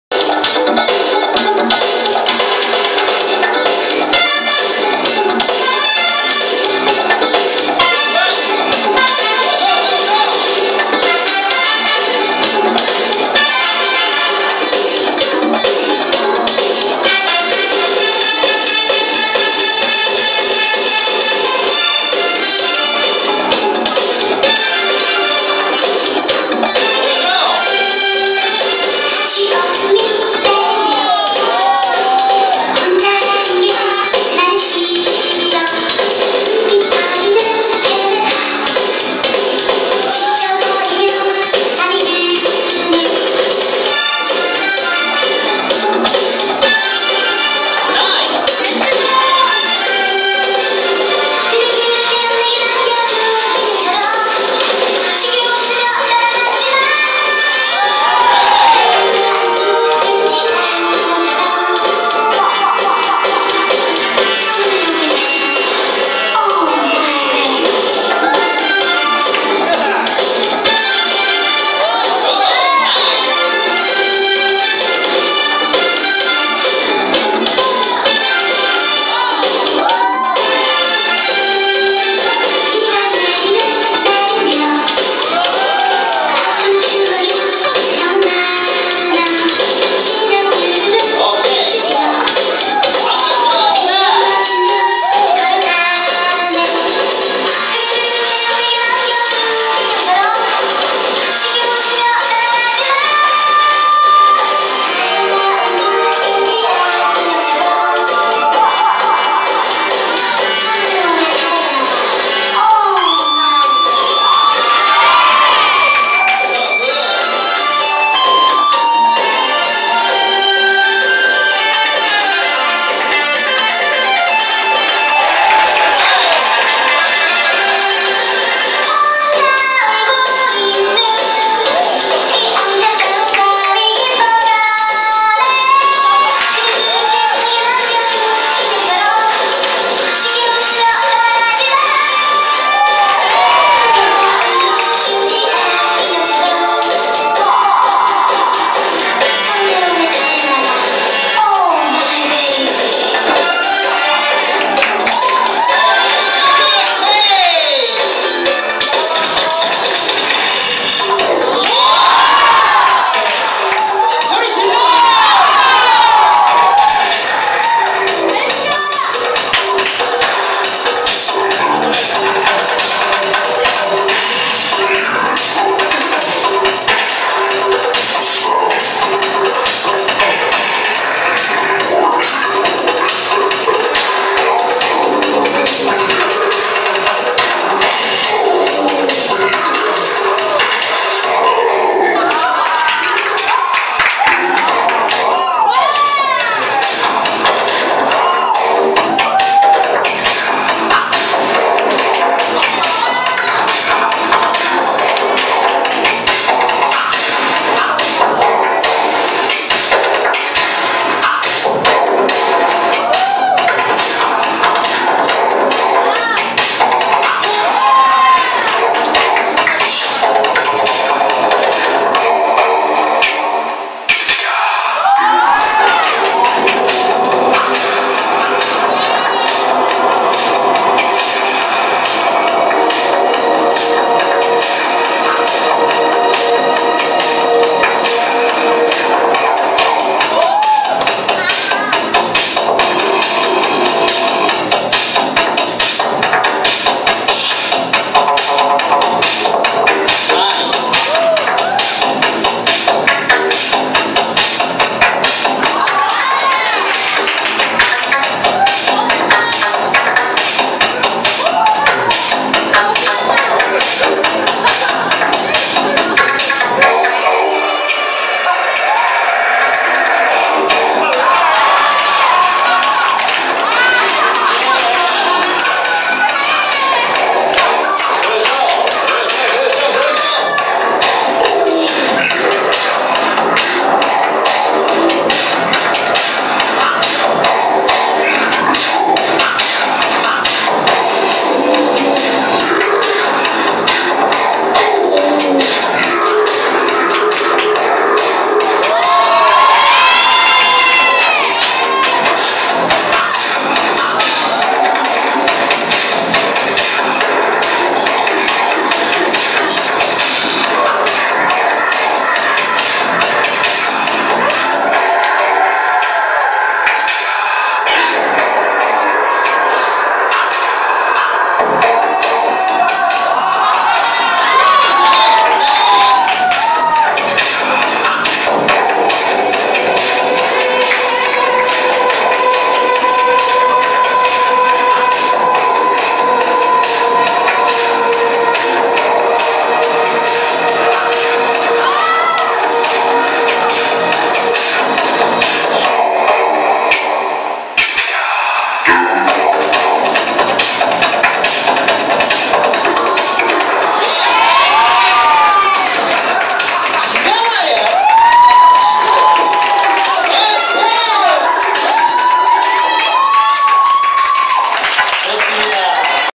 SUPER DANCER K [ WAACKING SIDE ] ROUND 7 [ FINAL ]
SUPER DANCER K [ WAACKING SIDE ]
9.15.SUN
JUDGE- HANS / JINI
MC - F.S MINGKOO
DJ - FUNKY MAN